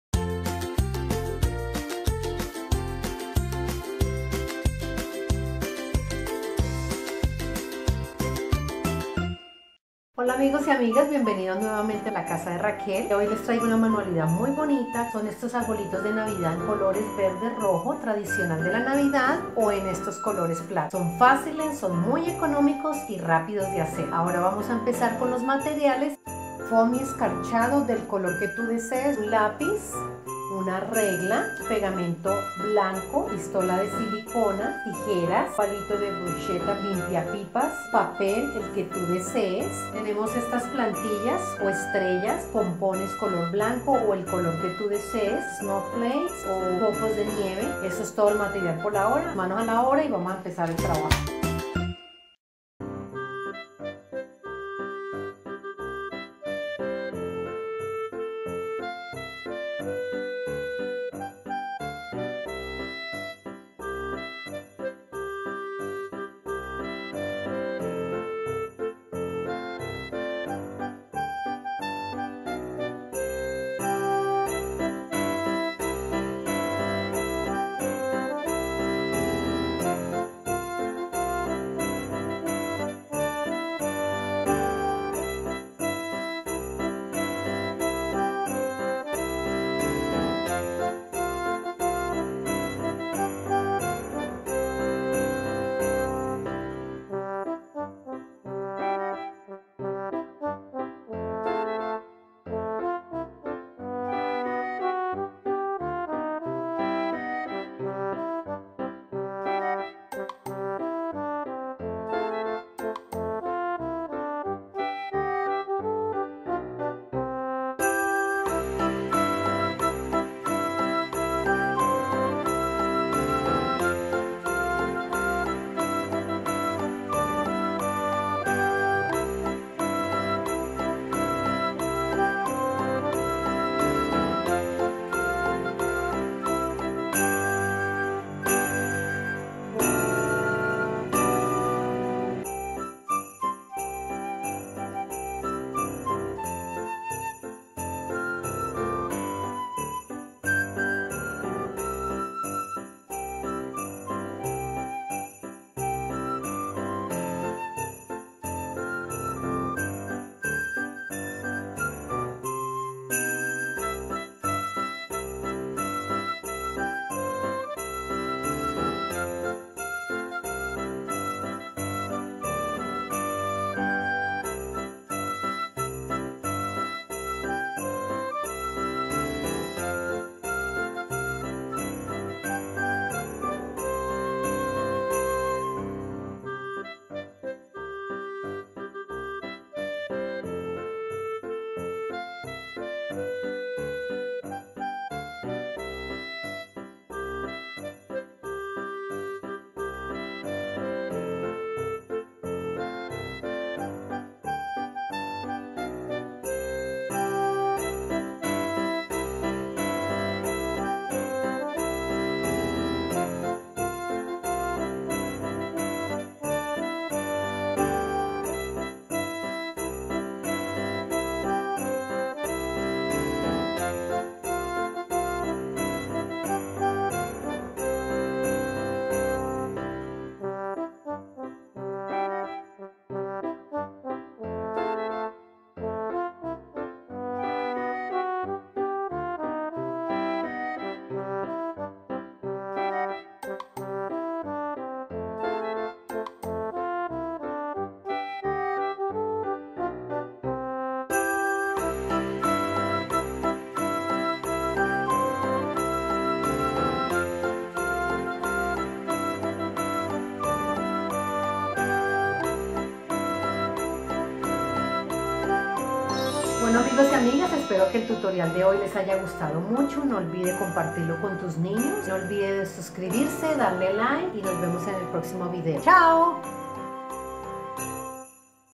Ropero Raquel 10. Mini arbolitos navideños con papel reciclado
Hola amigos en este vídeo te quiero compartir como realizar un bosque navideño con mini arbolitos de cartón para decorar en esta navidad . Es fácil Rápido y muy entretenido, acompáñame a realizarlo.  Aprovecho esta oportunidad para desearles feliz Navidad y Un prospero año nuevo salud y felicidad para todos. Chaoooooooooo.
MATERIALES.
Foamy escarchado
Lapiz Regla
Pegamento Blanco o Pegamento escolar.
Pistola de Silicona
Tijeras
Palito de Bruchetta
Limpia Pipas
Papel decorativo con diseños
Plantilla de Estrella (la puedes bajar de google. Buscas como patrón de estrellas).
Copitos de nieve y pompones blancos. (puedes usar todo lo que tengas para decorar los arbolitos).

Materiales
Copa de Cristal cualquier tamaño.
Un pedazo de icopor
Una hoja de foamy blanco
Decoraciones navideñas y hojas verdes
Un poco de nieve artificial o mini bolas de icopor
Cintas decorativas
Una vela de parafina de tamaño proporcional a la copa.

APROVECHO PARA DAR LOS AGRADECIMIENTOS A MIS COLABORADORES SON ELLOS QUE HACEN POSIBLE REALIZAR ESTOS VIDEOS.
Camara: Melina Grisales
Edición: Andrey Suta
                Steven Grisales.
Diseño de Pagina: Giovanny Gomez.
Asistencia de imagen y vestuario: El ropero Raquel Bogota Colombia.
Categoría
Instructivos y estilo